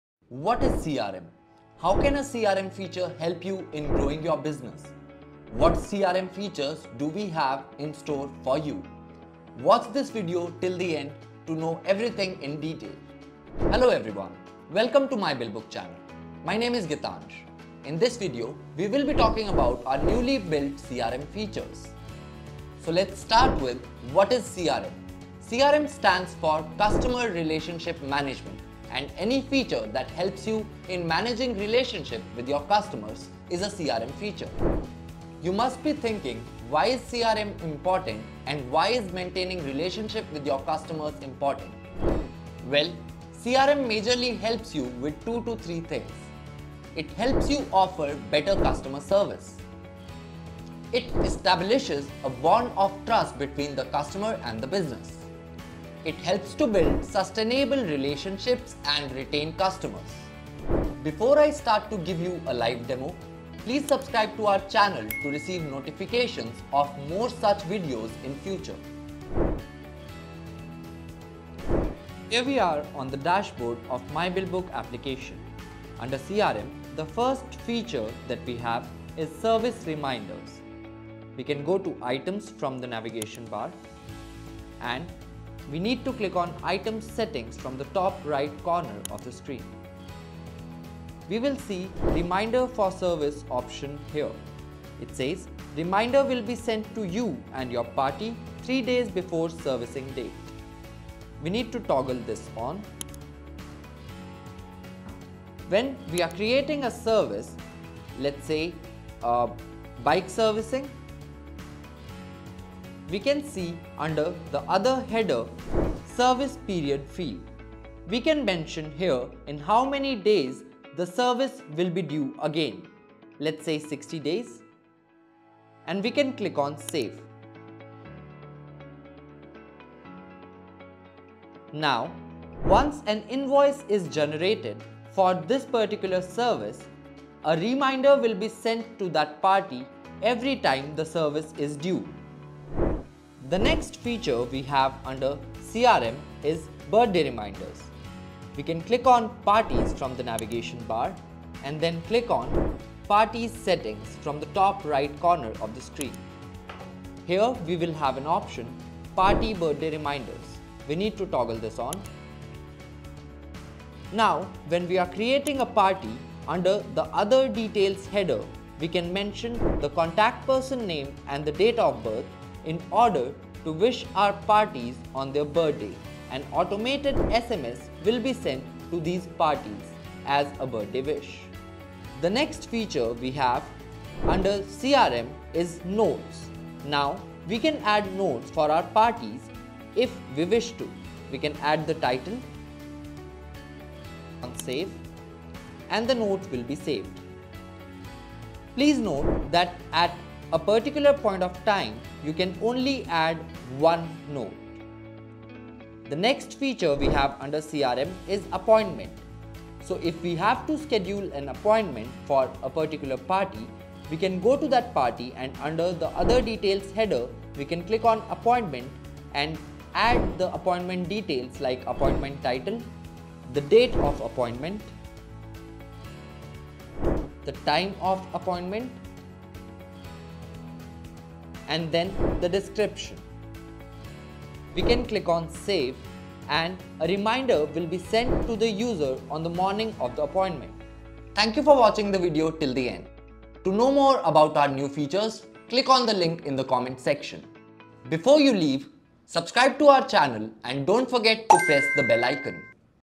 CRM in myBillBook Software |Customer Relationship Management | A Guide To CRM Software By myBillBook
Why do you need CRM?

CRM stands for customer relationship management and any feature that helps you in managing relationships with your customers is a CRM feature.

CRM helps you with a lot of things. Watch the video till the end to know it all.

Chapters:
0:00 - Introduction to CRM
0:38 - Advantages of CRM
1:11 - Live demo of CRM in myBillBook Software